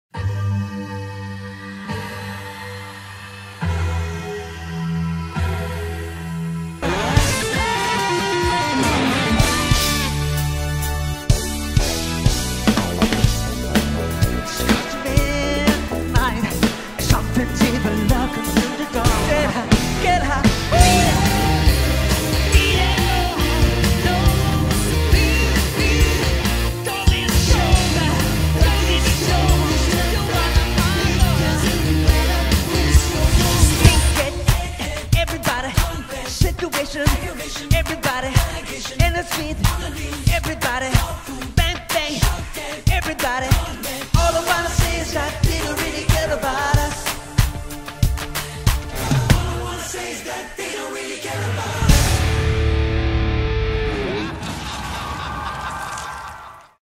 Sergio Cortés Michael Jackson
An engaging and dynamic show, Sergio Cortes doesn't cut corners, which is why he is considered The Best Tribute Show to Michael Jackson